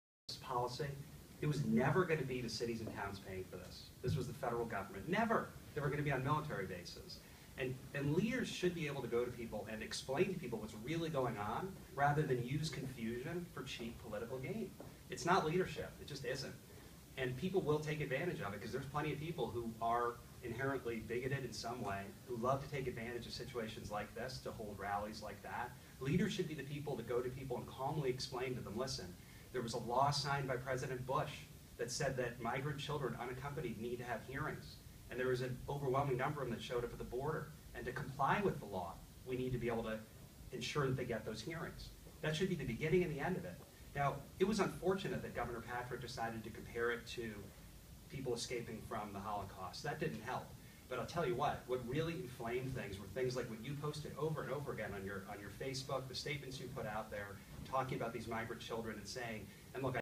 .@efalchuk accuses @JMacforgov of going for cheap political gain on immigration issue.
.@efalchuk accuses @JMacforgov of going for cheap political gain on immigration issue. @magov @mapoli